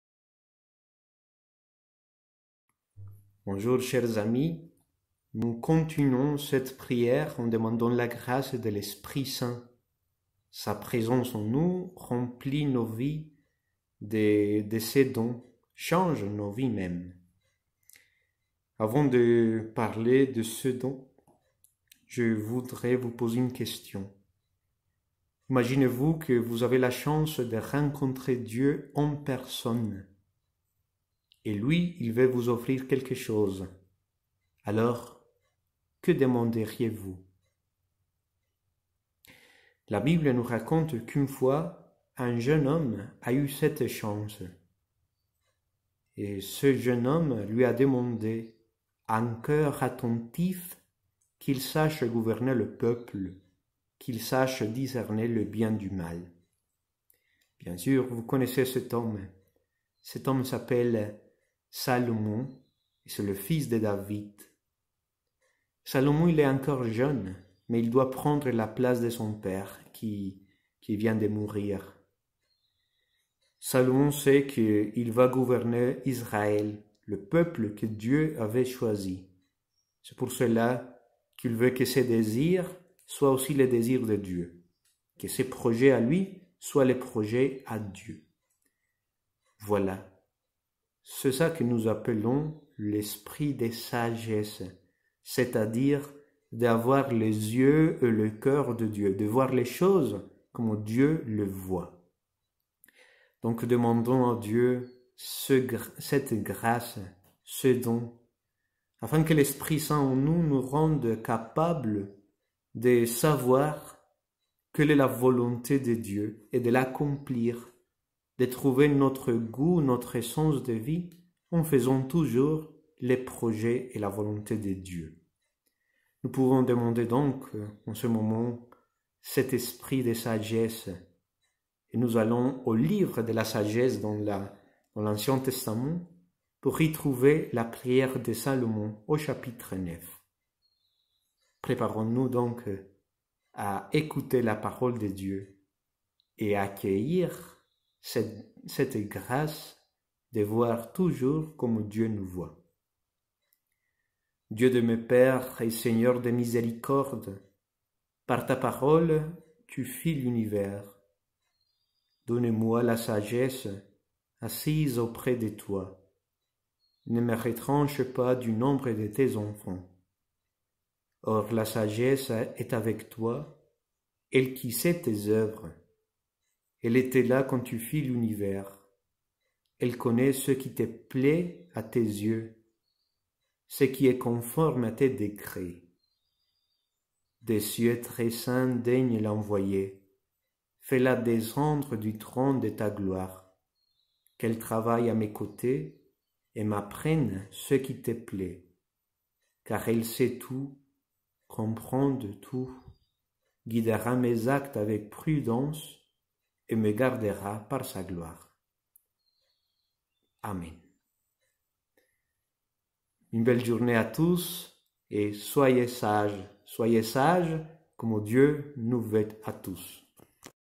Jour 2 - Témoignage de Juan David Arias
Si vous rencontriez Dieu en personne, que demanderiez-vous ?

Pour ce deuxième jour de neuvaine, le père Juan David Arias, vicaire pour l'ensemble paroissial de Pont-Sainte-Marie, vous pose cette question.